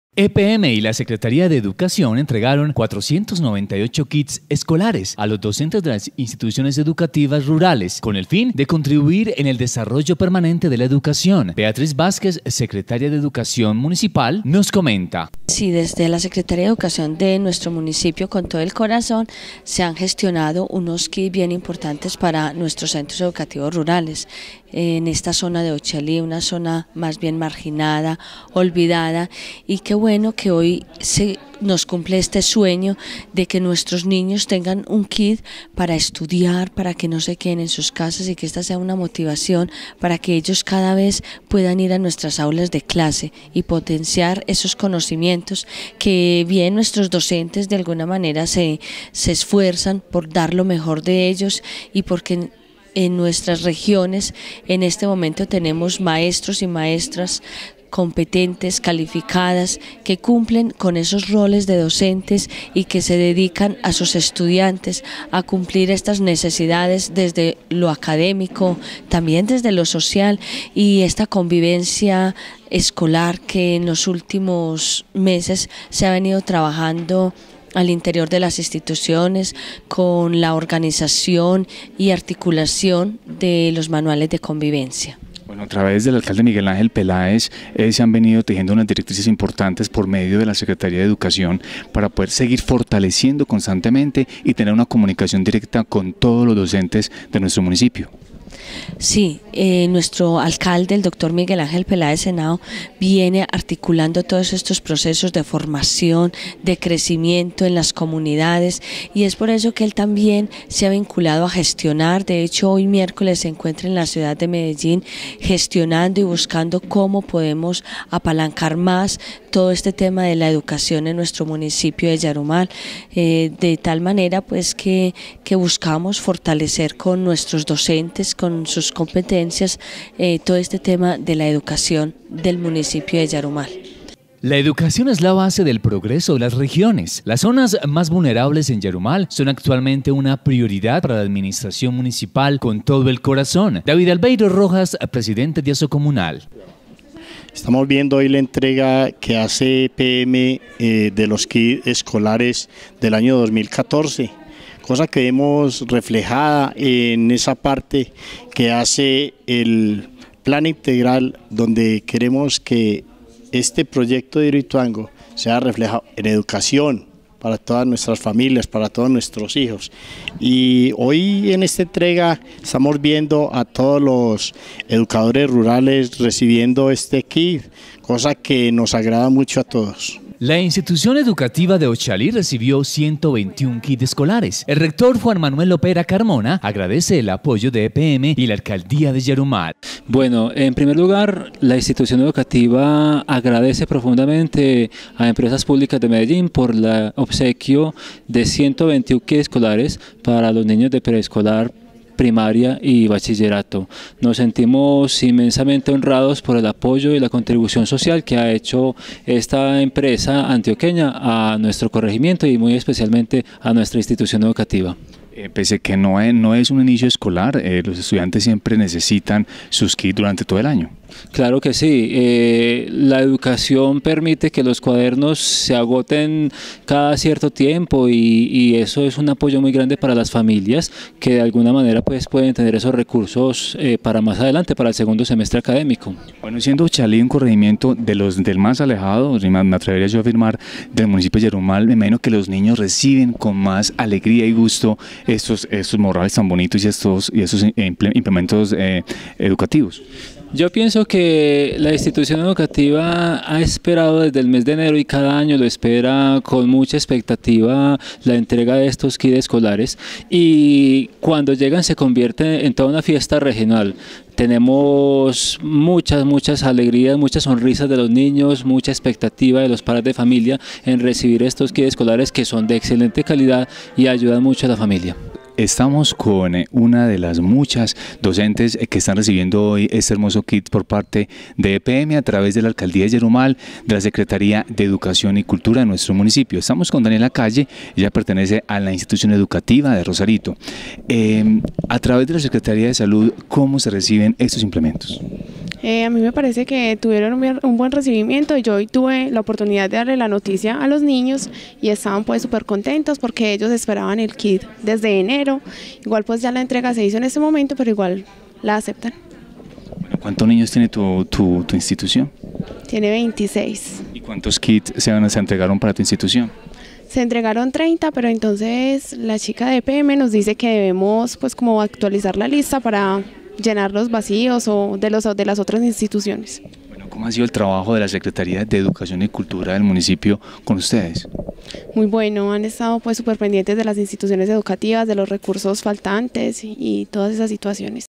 EPM ENTREGA KITS ESCOLARES A ESCUELAS DE LA ZONA RURAL
Epm y la Secretaría de Educación entregaron 498 kits a los docentes de las Instituciones Educativas Rurales con el fin de contribuir en el desarrollo permanente de la Educación. Beatriz Vásquez -- Secretaria de Educación.
La Educación es la base del progreso de las regiones. Las zonas más vulnerabes en Yarumal son actualmente una prioridad para la Administración Municipal Con todo el corazón. David Albeiro Rojas- Presidente de Asocomunal